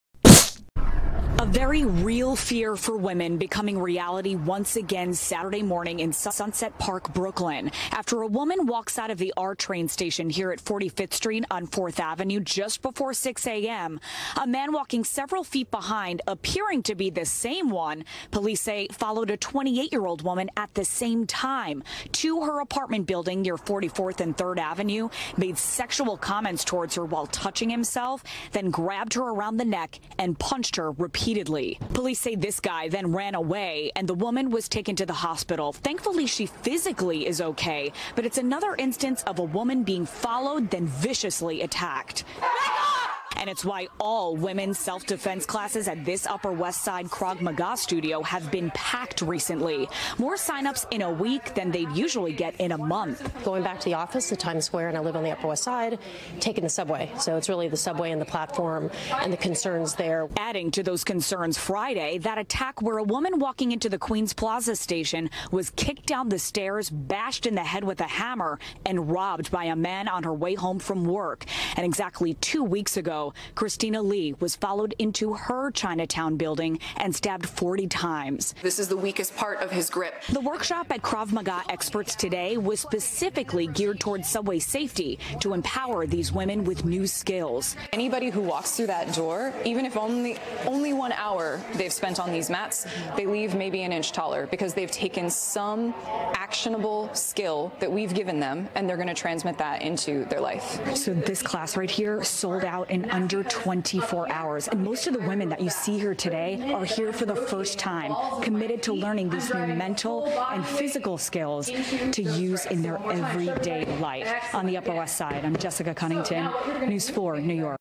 NYC Women Turn to Self-Defense Training Amid Subway Violence
Public Transportation Safety In NYC - NBC NEWS - Krav Maga Experts NYC
Women in NYC have decided to take safety into their own hands and be proactive. Training with Krav Maga Experts, Elaine Wilson and Tsahi Shemesh at the UWS, Manhattan.